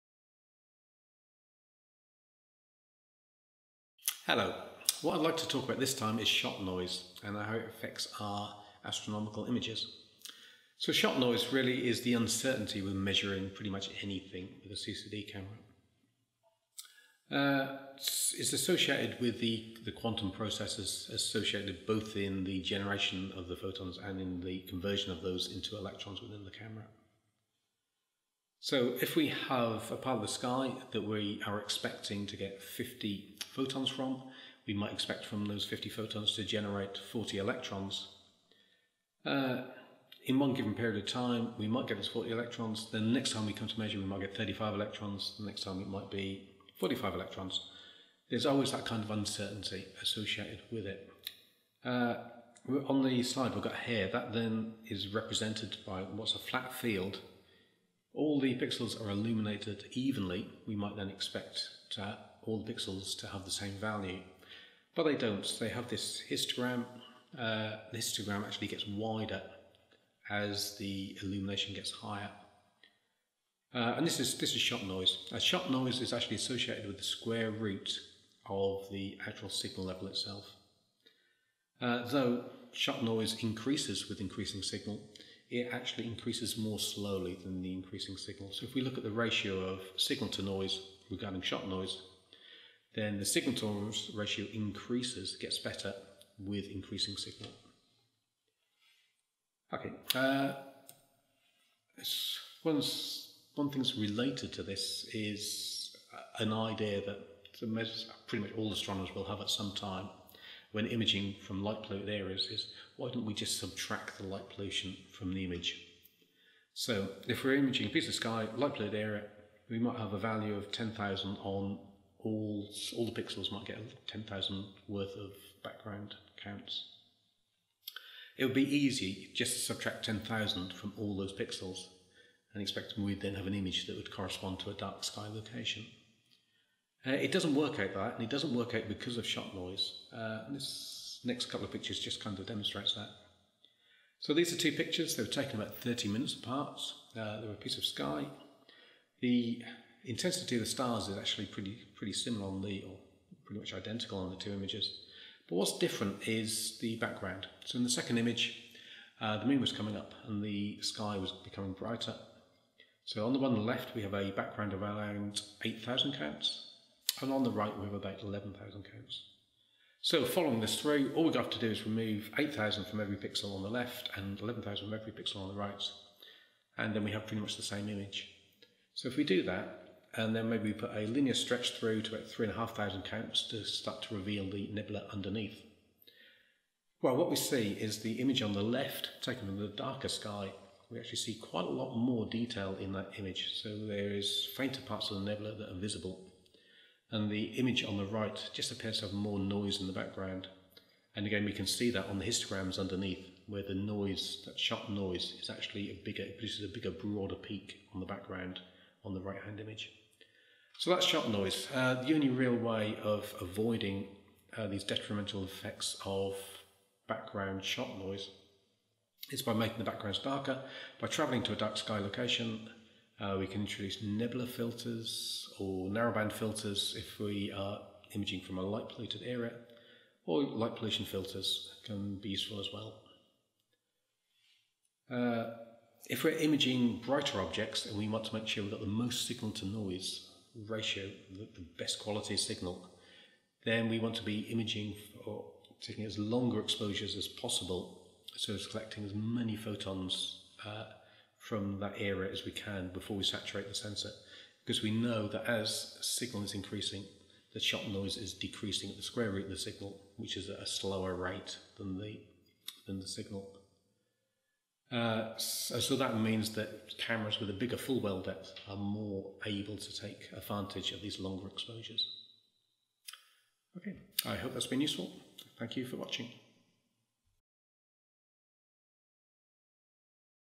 Shot Noise in CCD Cameras
Shot noise is associated with the uncertainty of measuring anything with a CCD Camera. Here, Steve Chambers takes a closer look at its relationship with signal to noise ratios, and what we can do to control it in our images. For a full transcript, please visit us